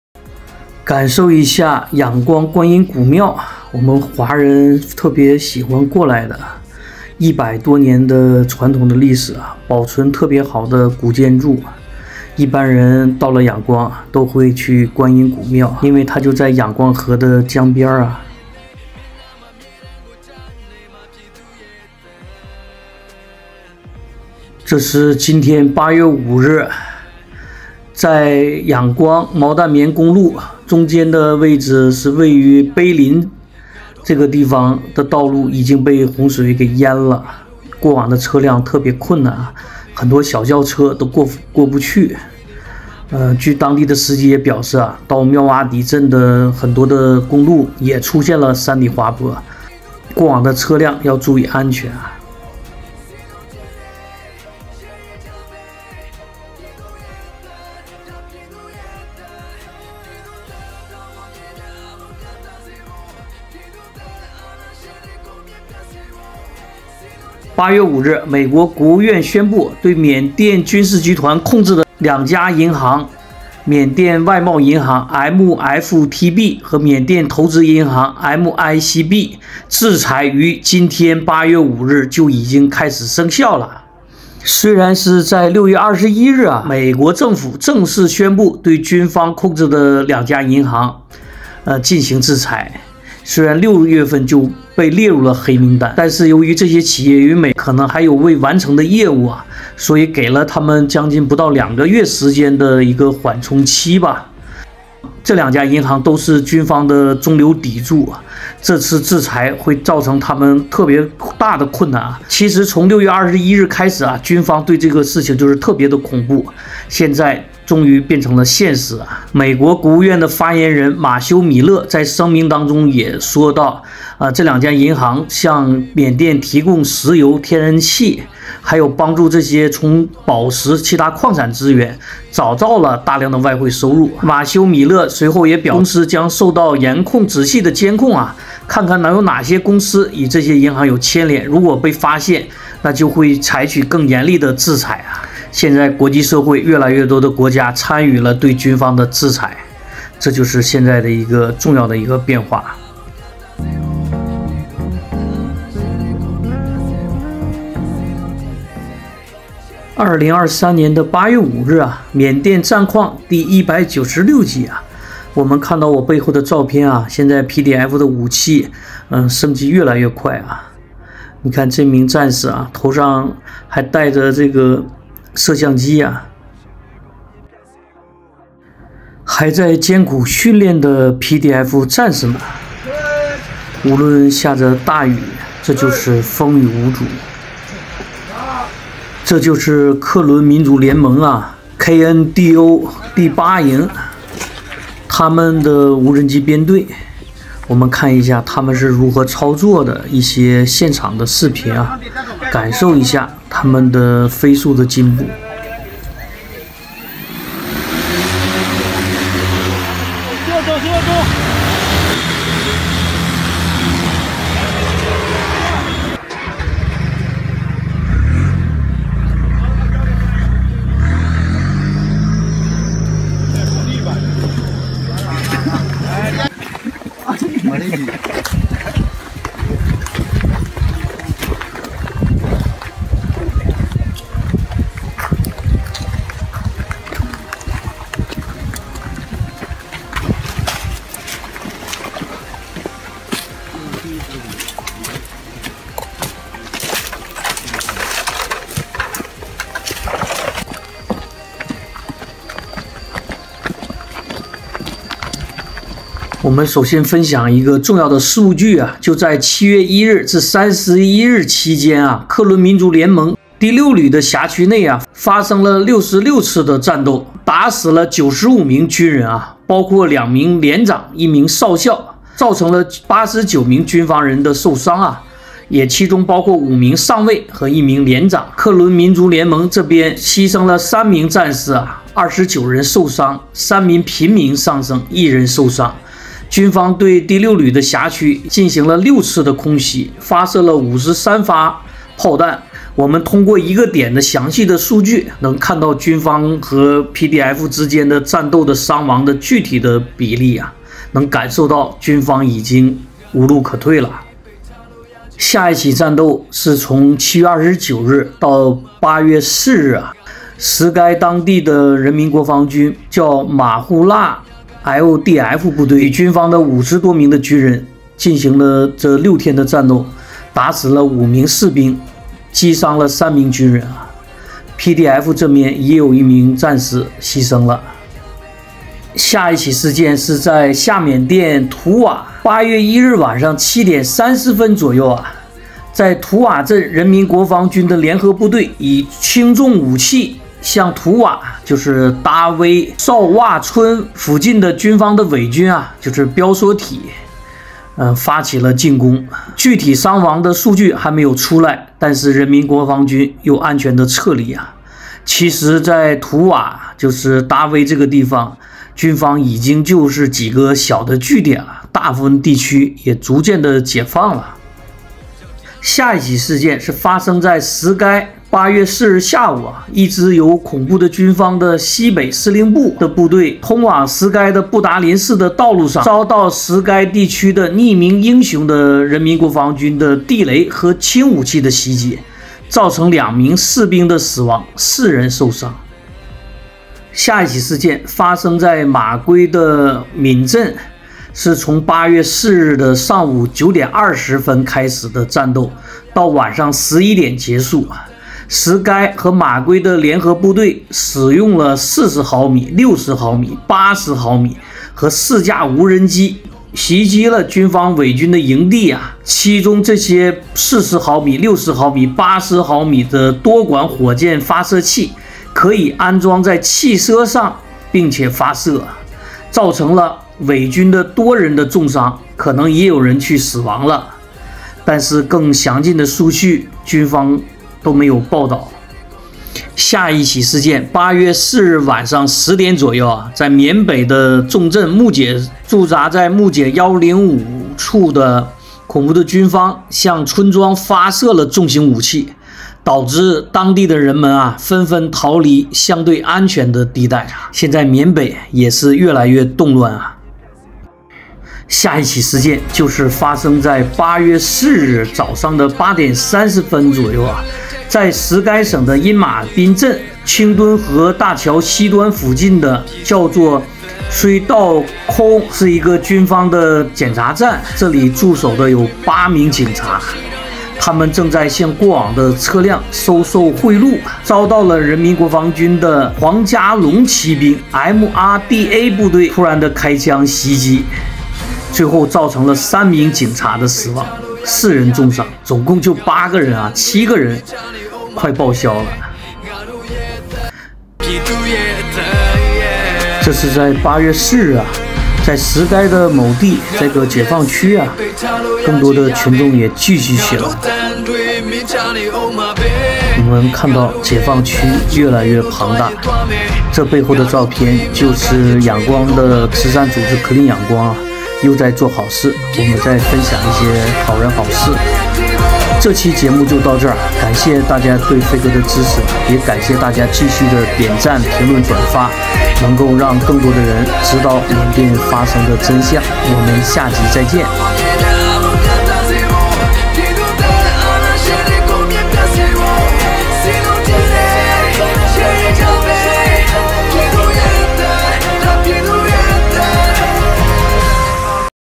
缅甸战况｜克伦民族联盟第六旅公布了辖区内7月份的战斗数据 仰光~毛淡棉公路被淹 【缅甸战况第196集2023年8月5日】
缅甸战况#飛哥緬甸説